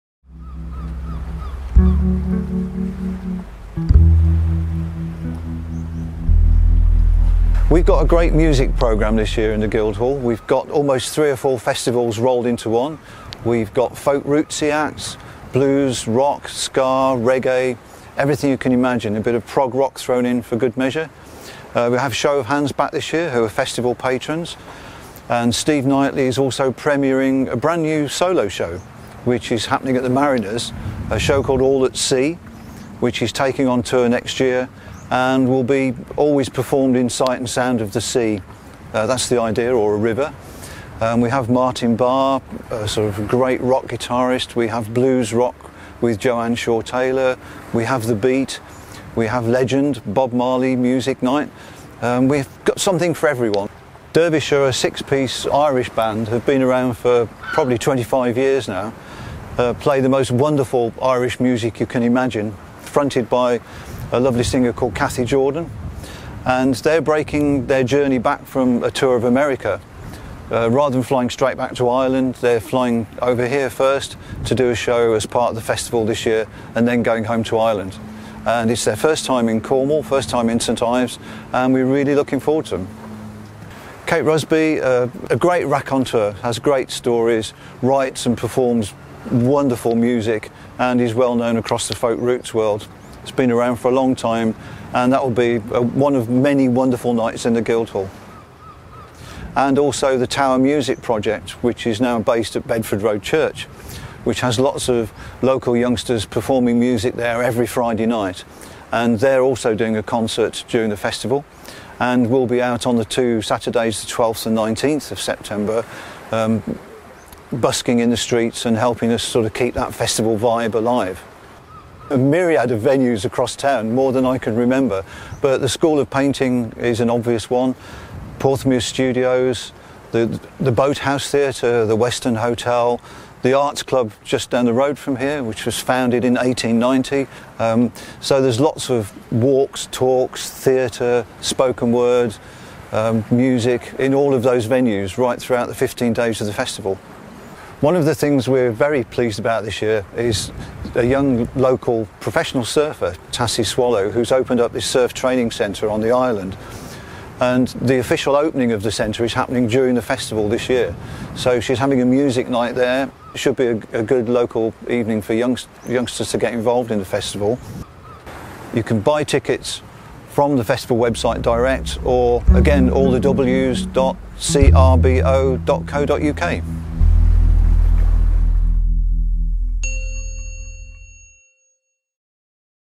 Presentation September Festival 2015
Dave Brotherton introduces us to St Ives September Festival 2015.